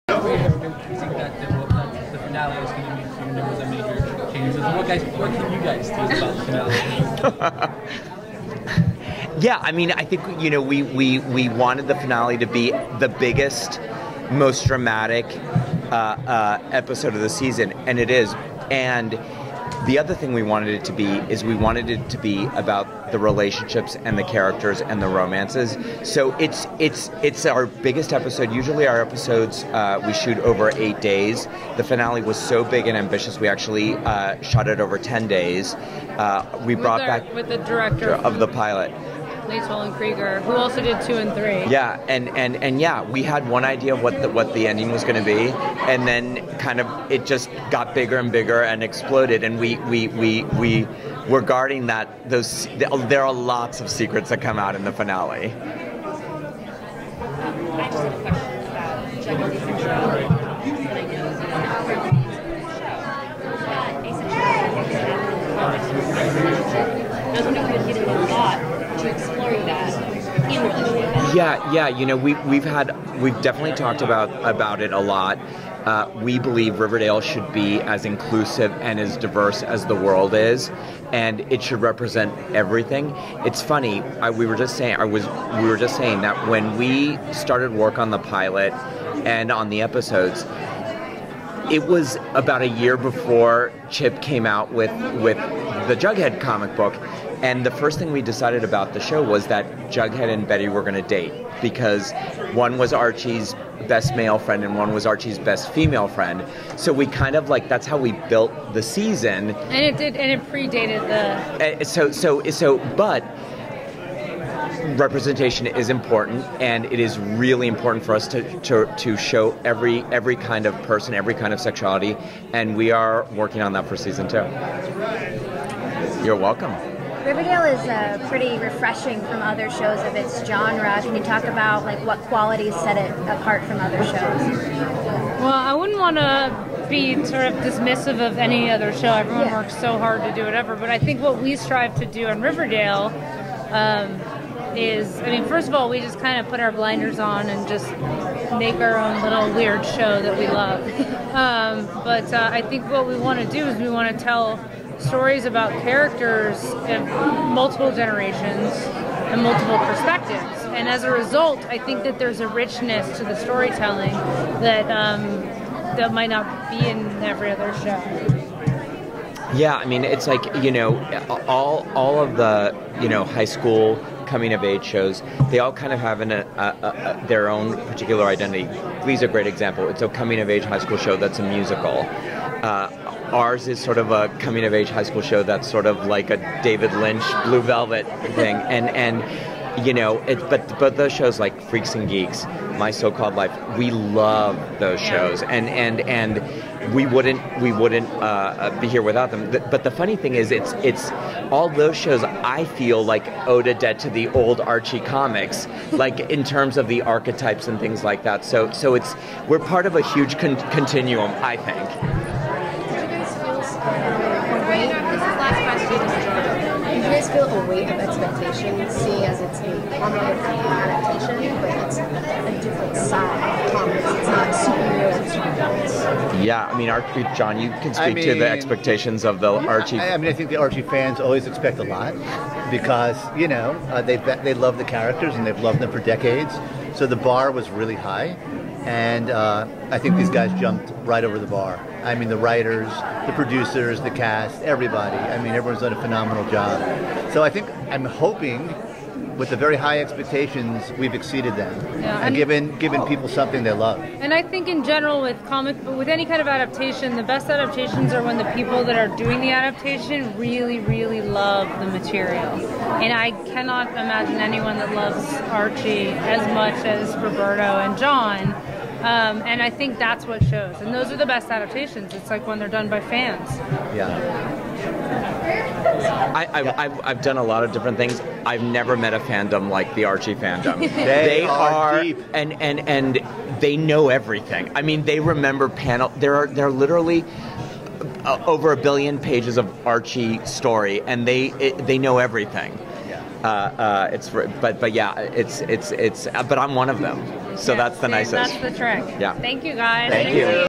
Riverdale Interview with Executive Producers at Wondercon
Interview with Riverdale's Executive Producers Sarah Schechter, Roberto Aguirre-Sacasa & Jon Goldwater at 2017 Wondercon!